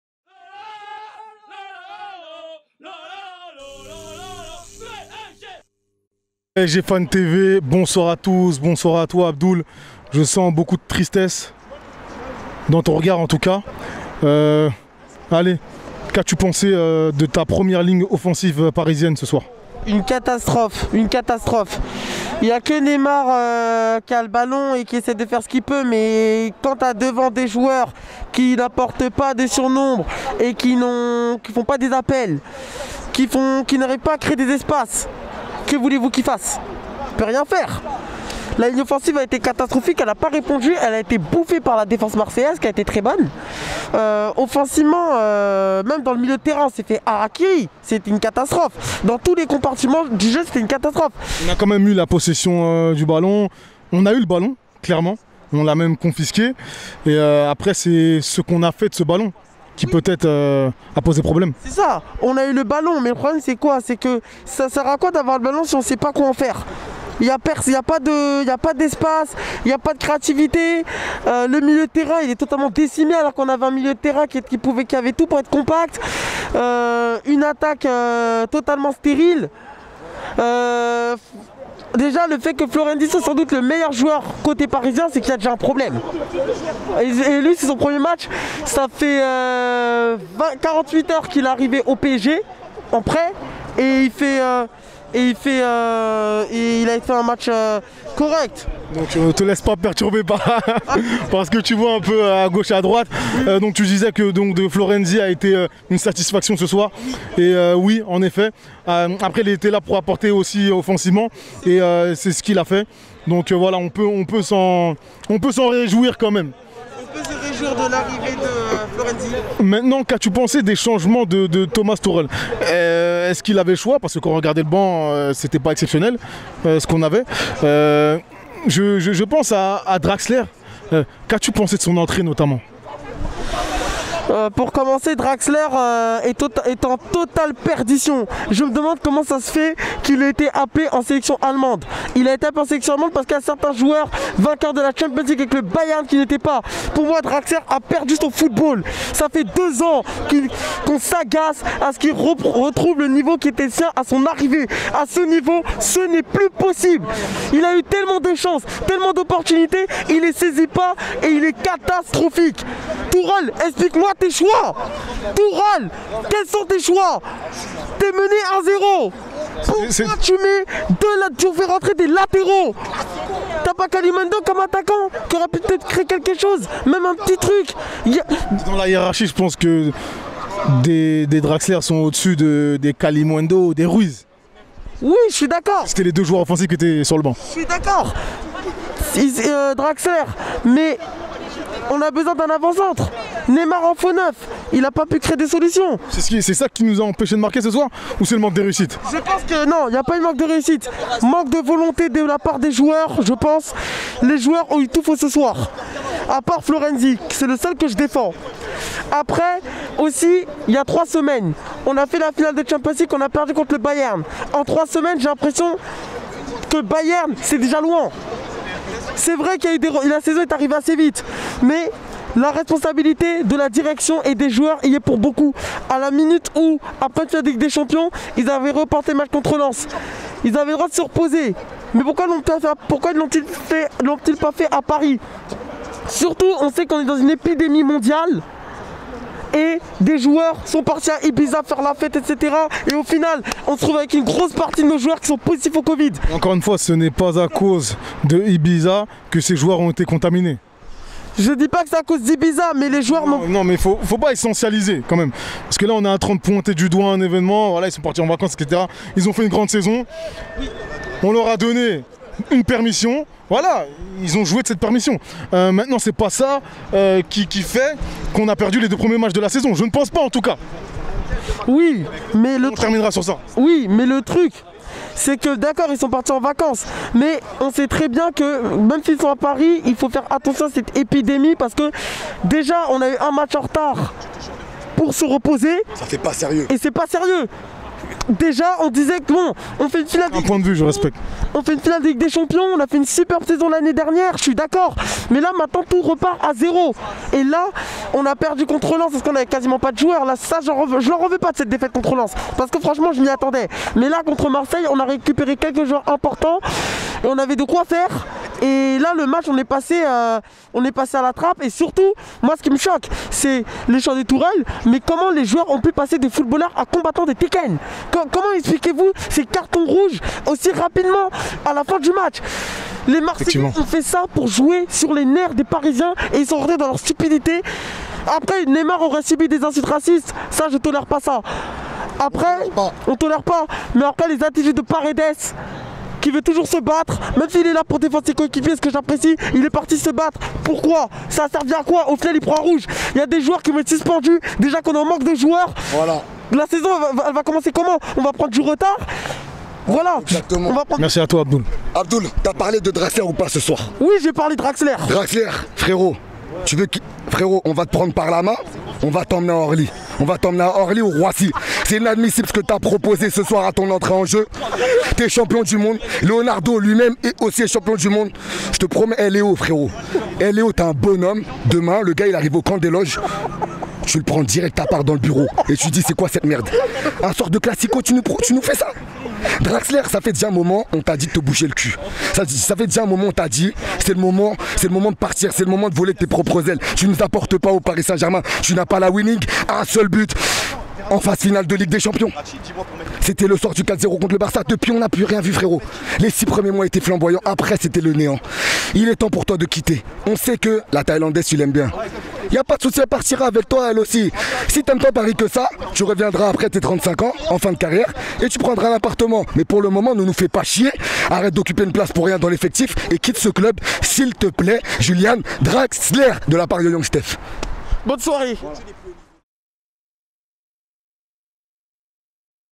PSG vs Marseille 0-1 | Draxler A Perdu Son Football ! (Abdoul)
Les supporters parisiens analysent la rencontre du Paris Saint-Germain face à l'Olympique de Marseille lors de la 3ème journée de Ligue1. C'est la deuxième défaite de la semaine, qui vient clôturer une soirée mouvementée. Trois joueurs parisiens ont été expulsés, vivement mercredi prochain...

C'est Parti !!